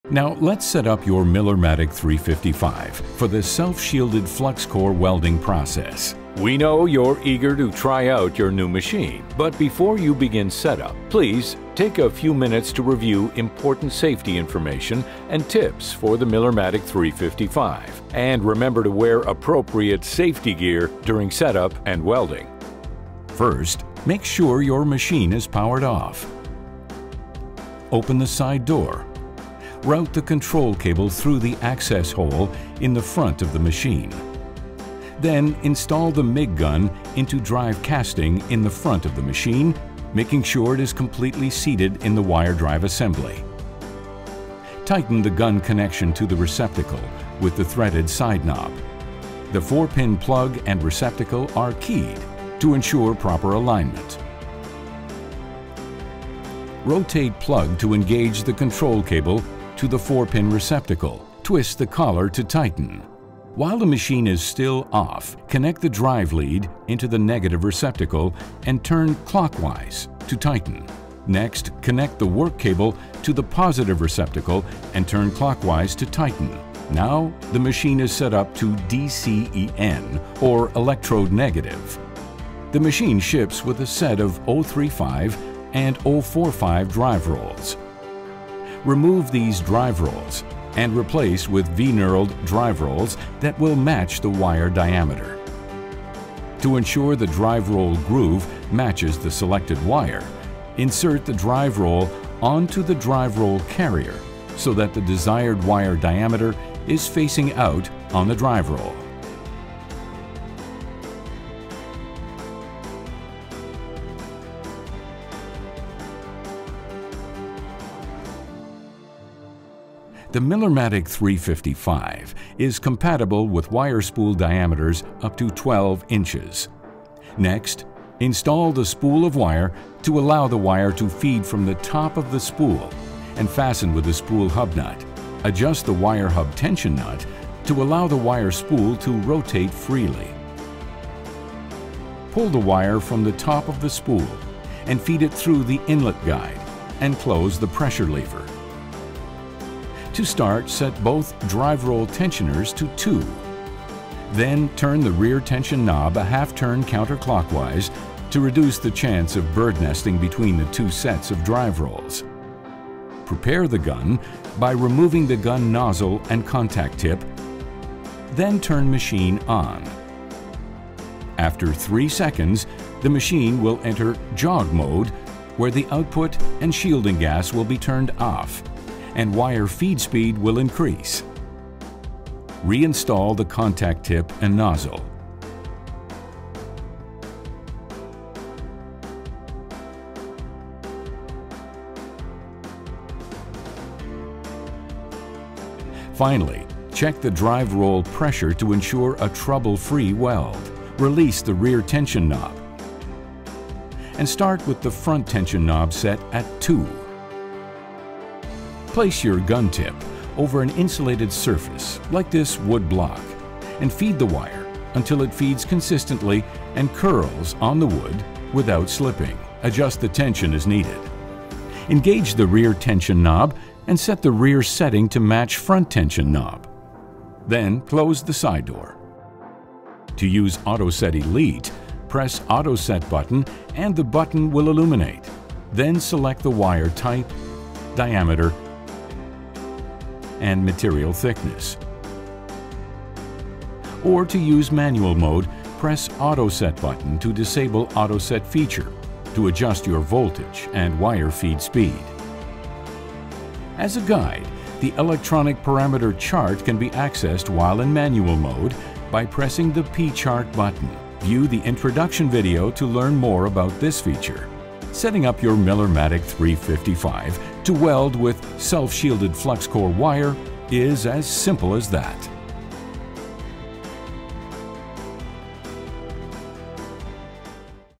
Setting up Self-Shielded Flux Cored | Millermatic® 355
Learn how to set up your new Millermatic® 355 welding power source for self-shielded flux-cored welding.
This video shows how to install a MIG gun and set up the wire drive assembly to assure smooth wire delivery. You’ll also learn how to connect the gas and set the machine’s parameters utilizing the electronic parameter chart, manually or Auto-Set — everything you need to start flux-cored welding.